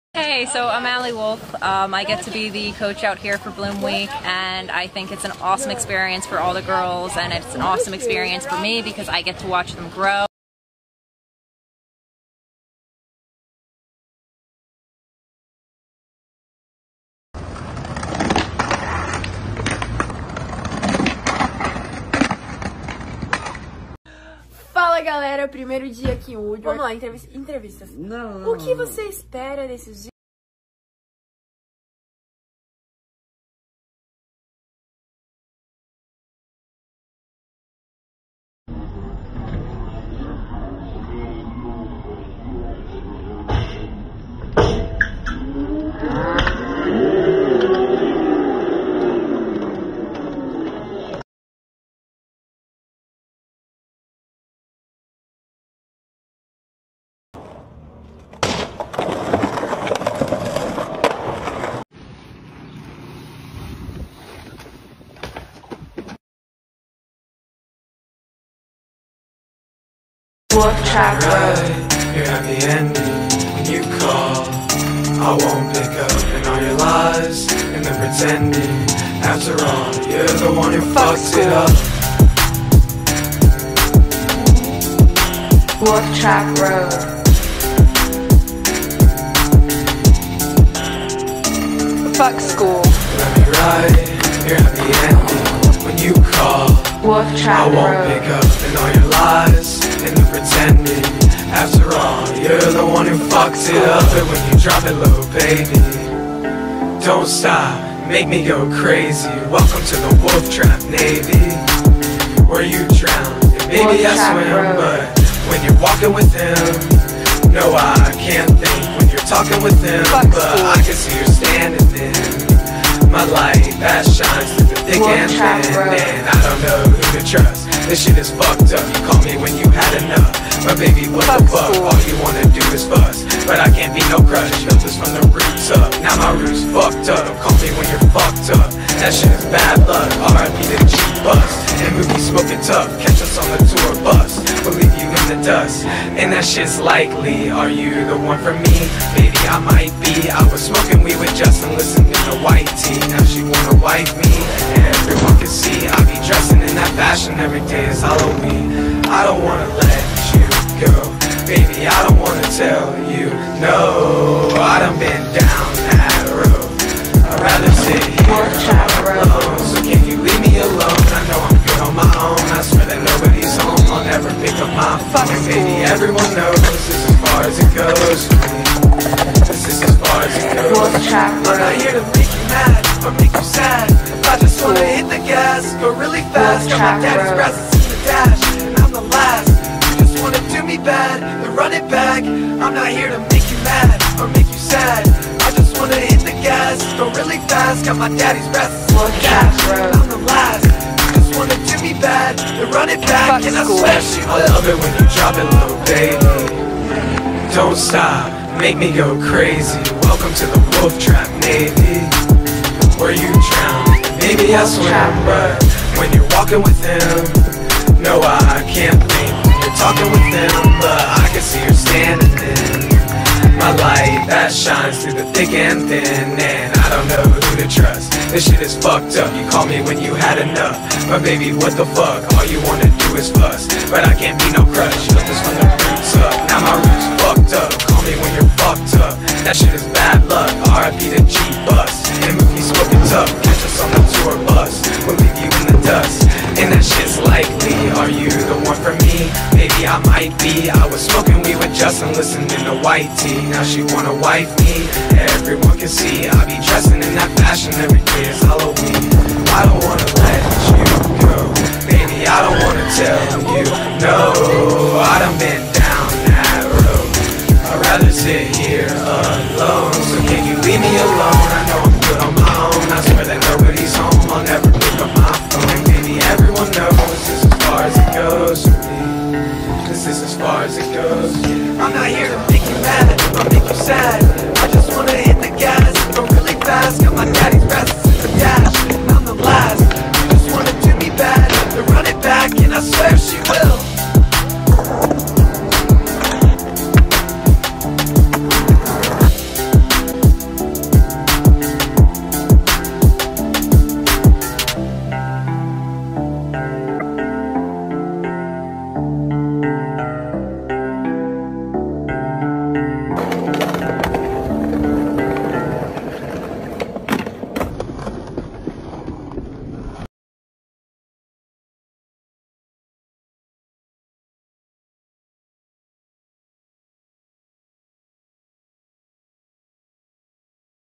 Vans Barge The Locals
Vans Flow Team in the East, They have so much footage of the team going to eastern spots and You can look for the Locals getting on their boards

This song is by WolfTrapRoad

Appearances
Chase Dehart, Giovanni Vianna of Vans skateboarding Brasil, Vans Skateboarding Canadian Etienne,Teresa Fernandez Miranda of Vans Europe  Vans Bmx pro Angie Marino, Chase Lenox Snyder, Allie Wolfe,Ruby Lilley,Vans Skateboarding Pro Tyson Peterson, Broc Raiford,Ruby Lilley,Kyota Umeki of Vans Japan, Shiloh Catori,Vans Skateboarding Pro Daniel Lutheran,Kevin Shealy, Vans Bmx Brazilian Duda Pennso and Keir Sirlin

Go to the Vans Account find more videos like these